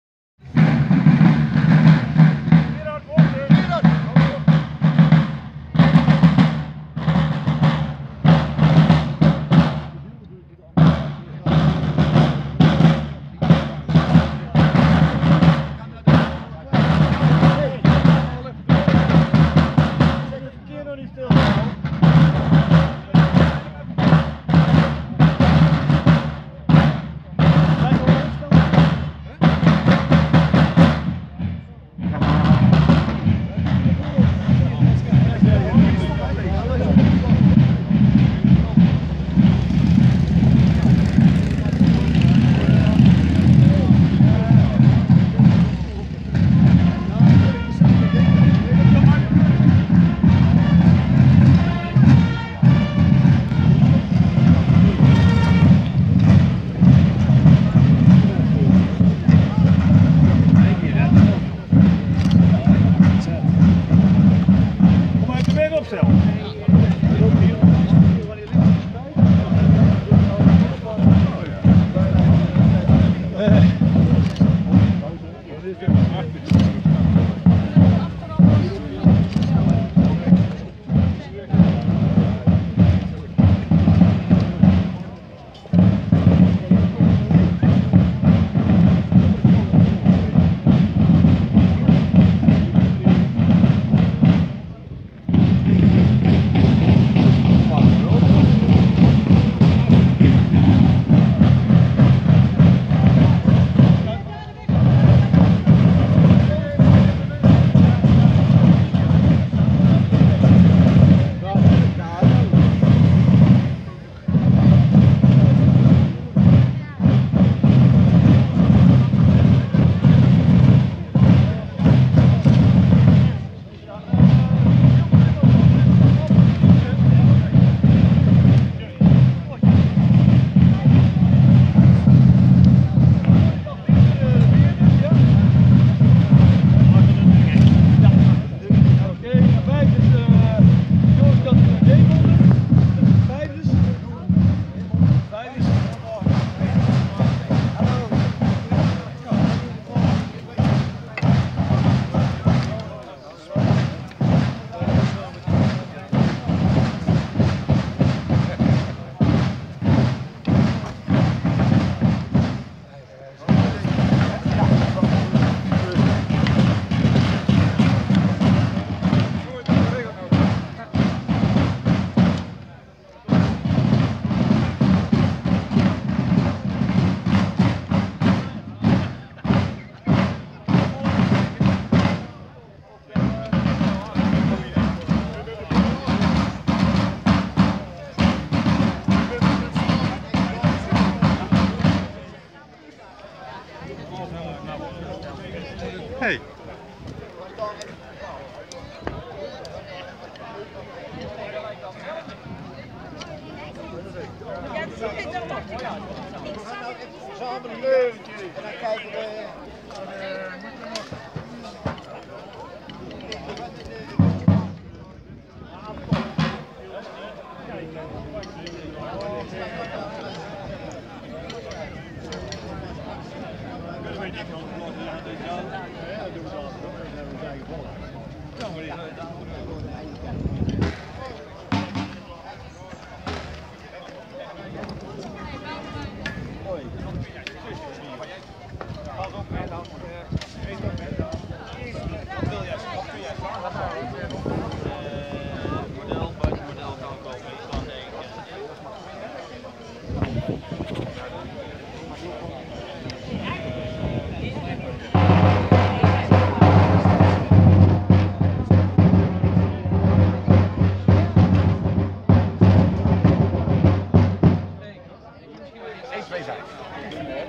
Kringgildedag Dommelgroep Sint-Michielsgestel
Vandaag hielden acht van de negen gilden  uit Den Dungen, Sint-Oederode, Vught, Boxtel, Liempde en 2 uit Gemonde gelegen aan de rivier "De Dommel" hun jaarlijkse Gildedag in Sint-Michielsgestel. Wegens overlijden van gildebroeder Peter Vermeer was Schijndel niet aanwezig. Op weg naar het gildeterrein maakte ik dit filmpje van echt Brabantse cultuur!